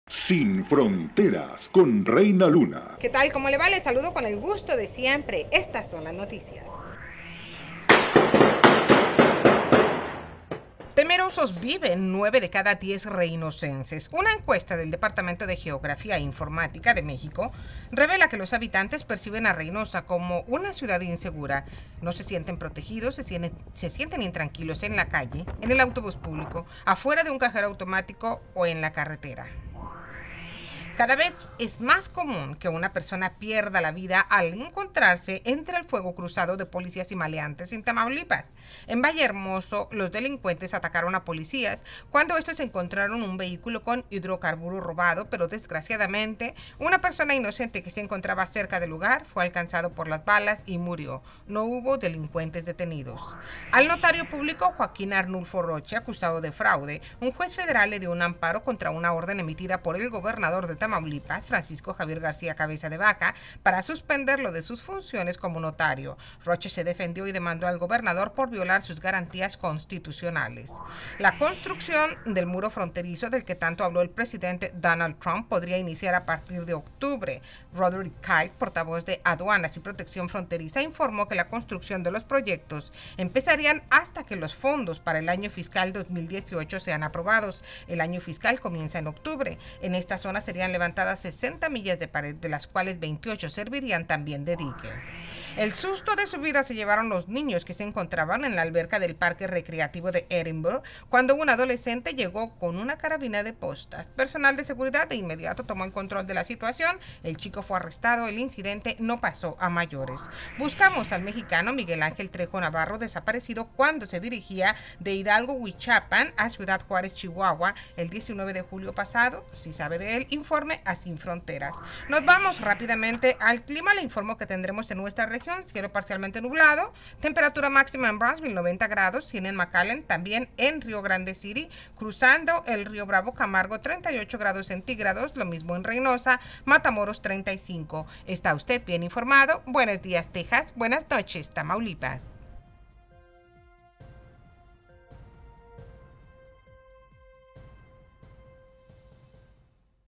Inseguros, nueve de cada 10 Reynosenses...
Cada vez mueren más inocentes en fuego cruzado policía-delincuentes...Demanda Notario al Gobernador de Tamaulipas... En Octubre iniciaría el muro fronterizo... Adolescente arrestado por llegar carabina a alberca pública... Buscamos al mexicano Miguel Ángel Trejo, desaparecido en junio pasado...